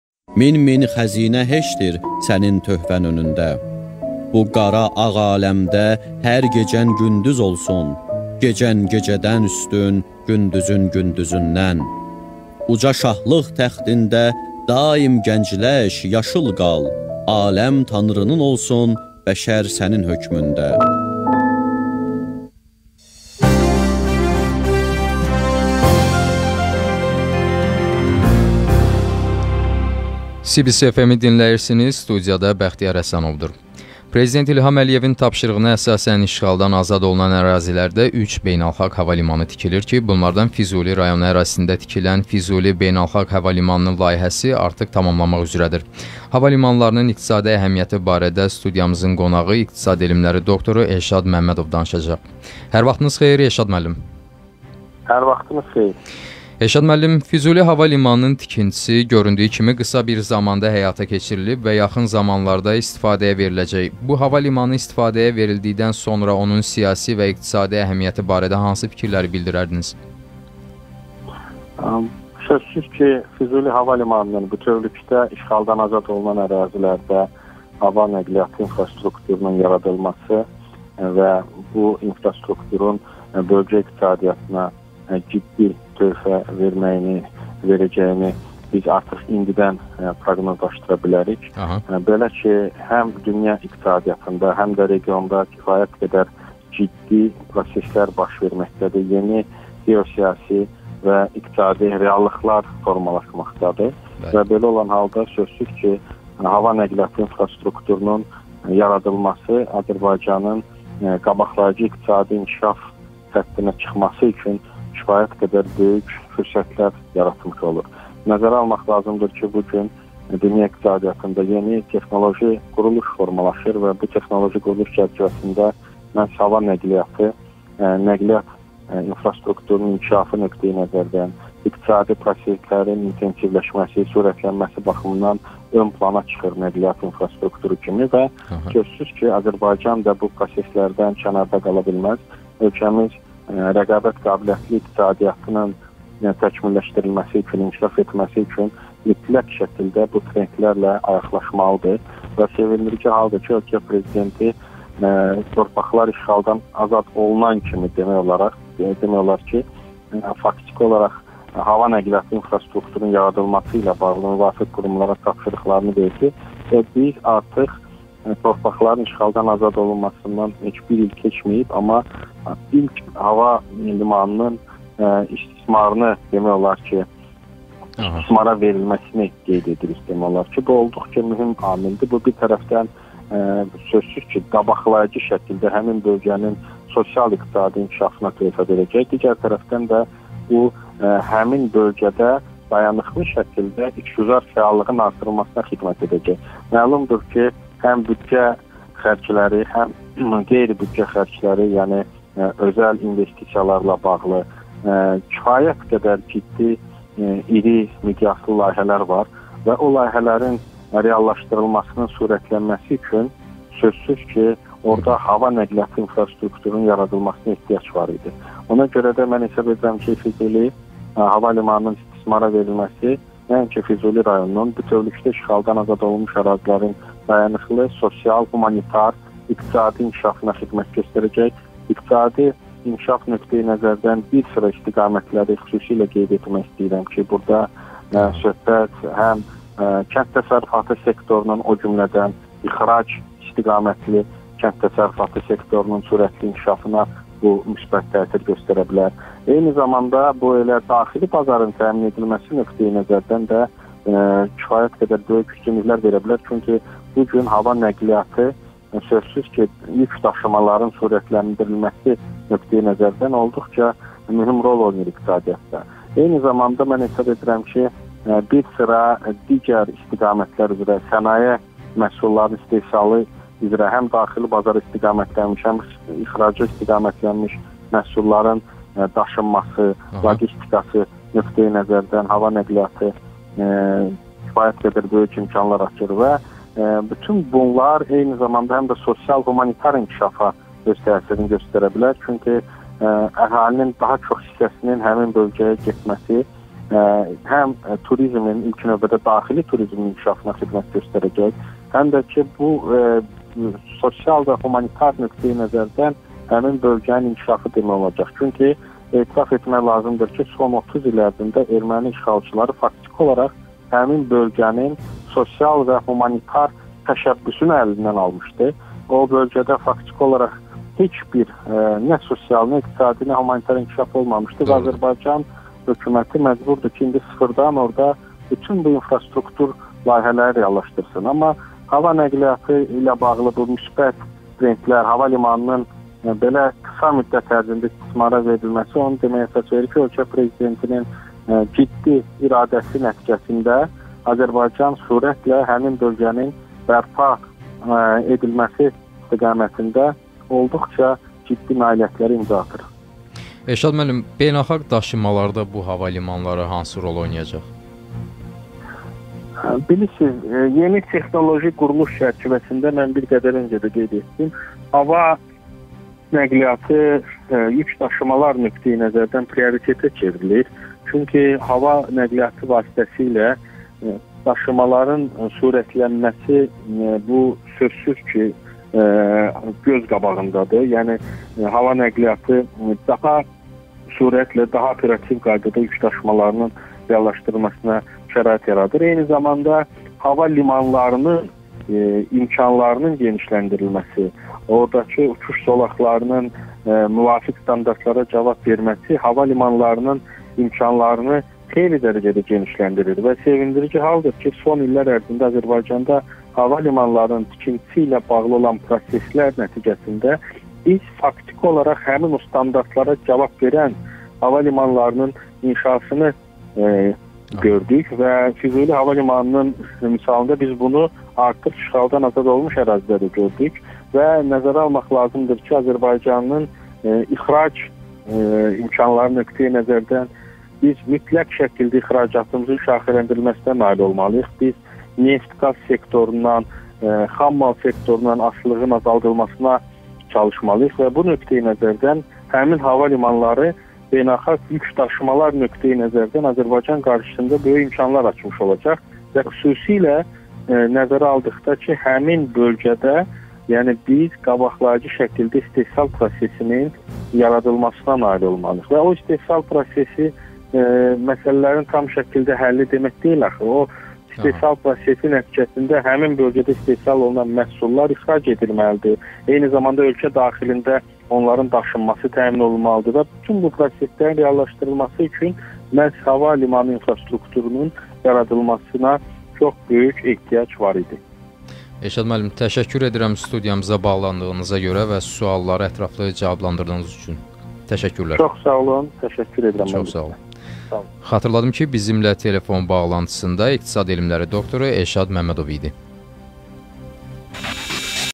İşğaldan azad olunmuş ərazilərdə üç beynəlxalq havalimanı tikilir
Havalimanlarının iqtisadi əhəmiyəti haqqında studiyamızın qonağı iqtisad elmləri doktoru Elşad Məmədov danışır